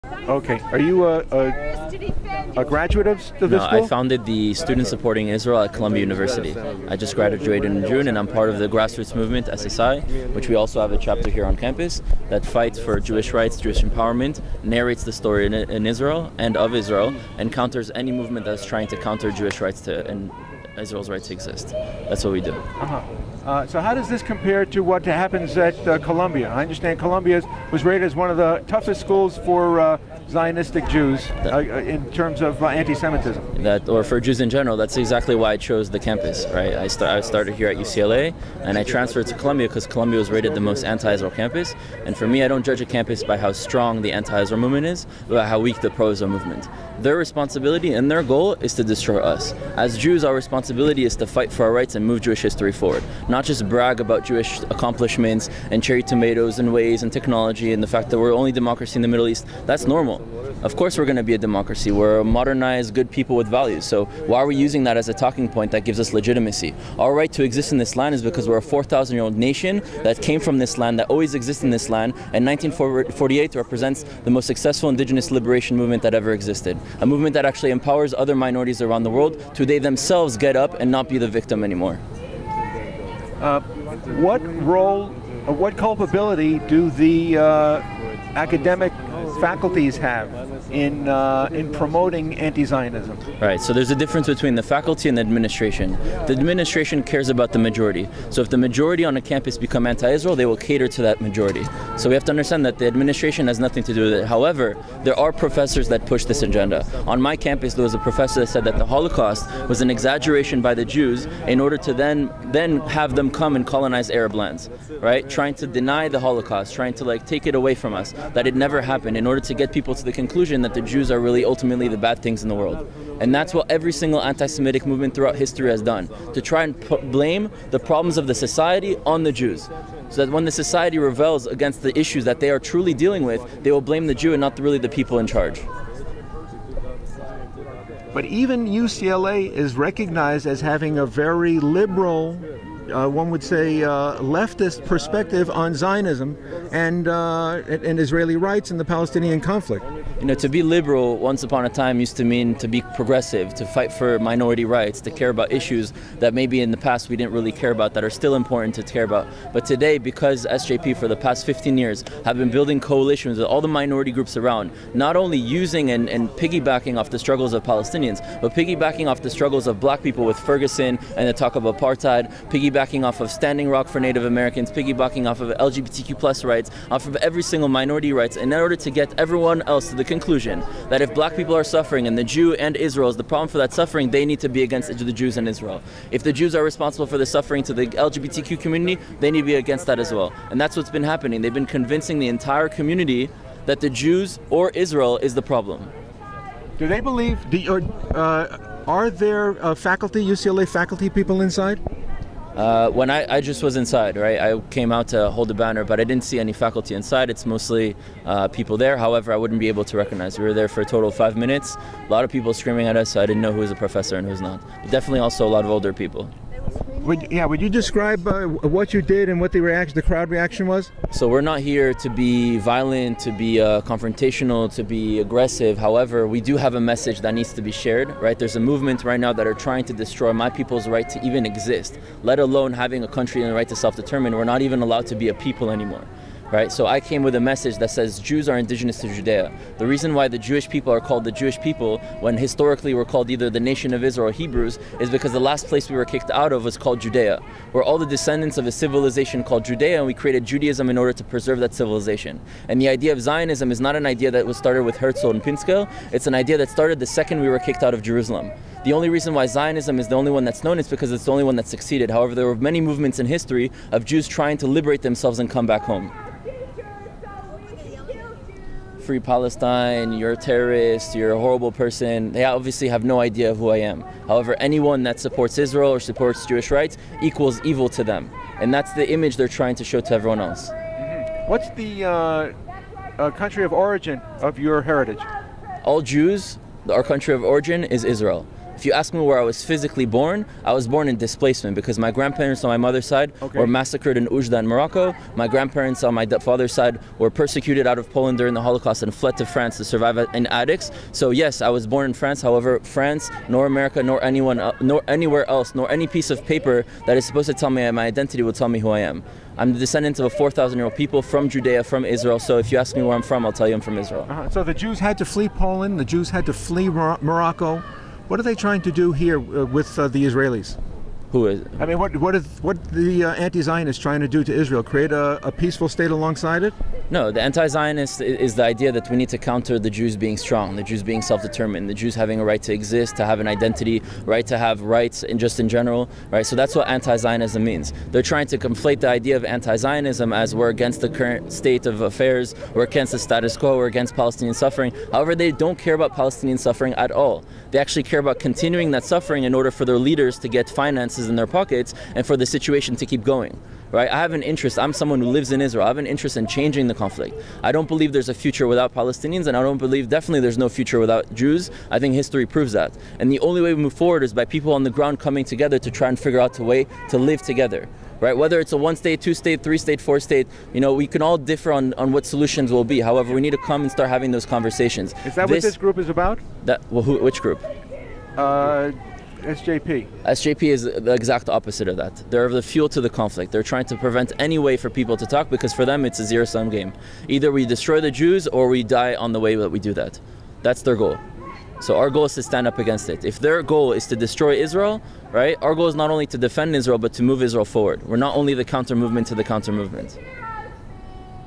UCLA Zionist defenders avenged Students 4 Justice in Palestine's trashing by crashing their NatlConf
Rudy Rochman, founder of Coumbia Univ's "Students Supporting Israel" counters "Students for Justice in Palestine" convention at UCLA the obvious history that Jews living in Judea can't be considered occupying as they are the INDIGENOUS people of Judea.